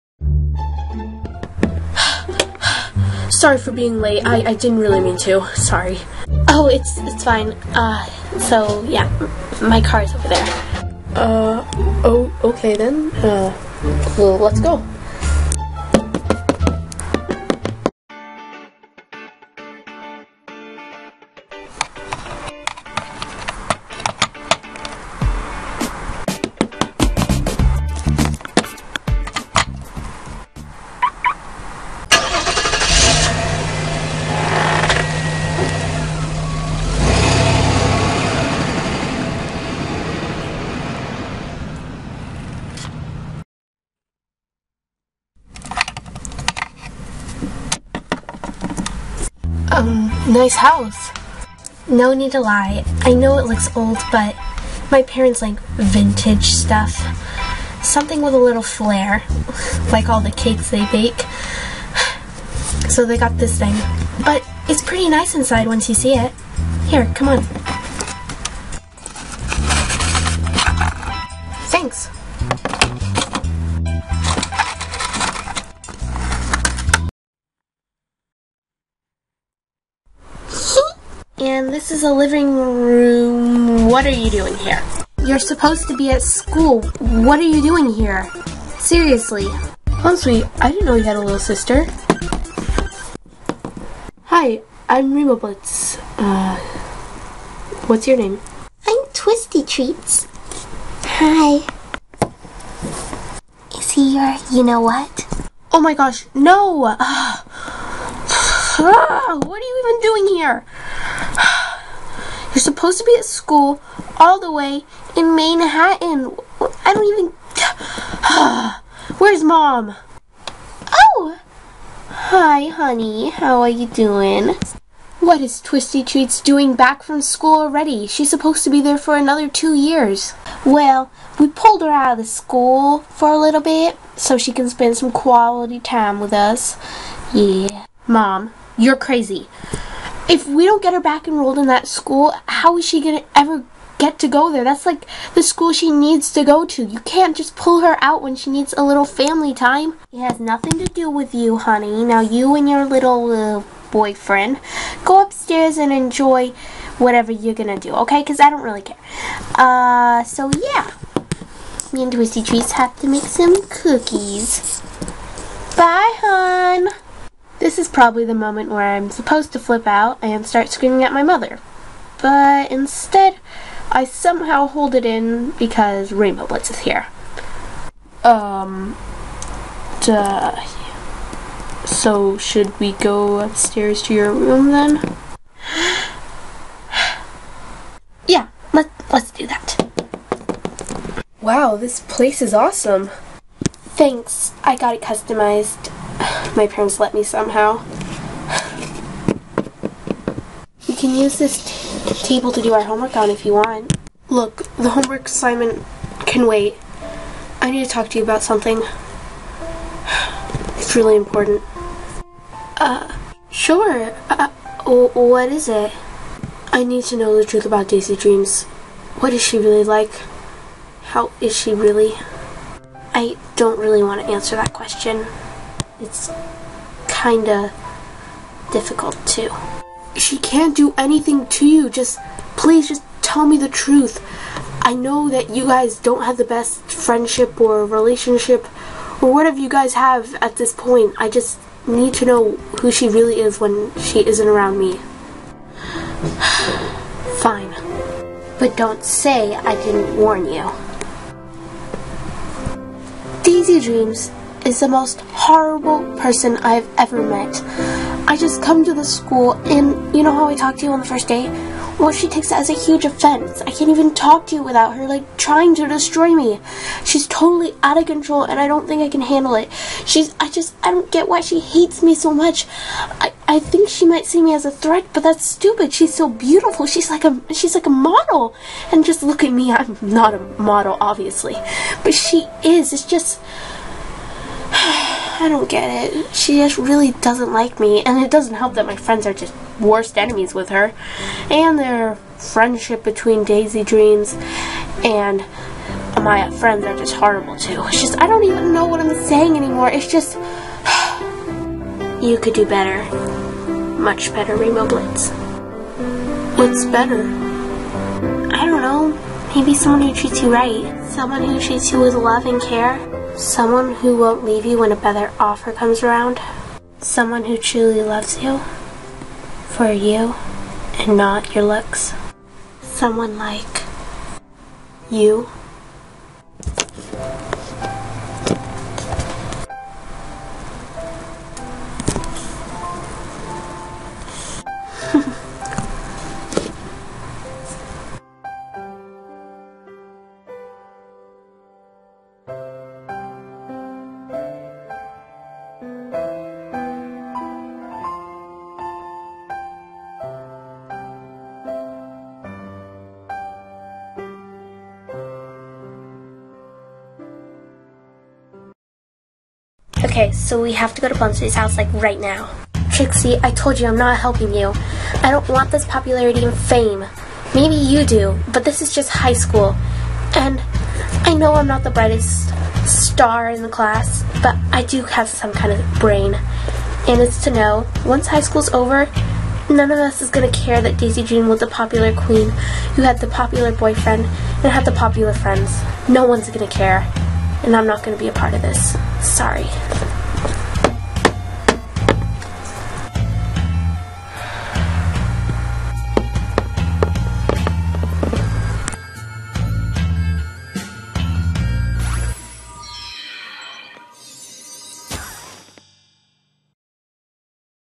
MLP New Girl ~ S2 EP6 *Highschool ends and no one cares*
Plumsweet has rainbow blitz over for a math project and something more happens. Rarity is done with dasiy dream and trixie. What will happen next time??
Stay with my channel to find out!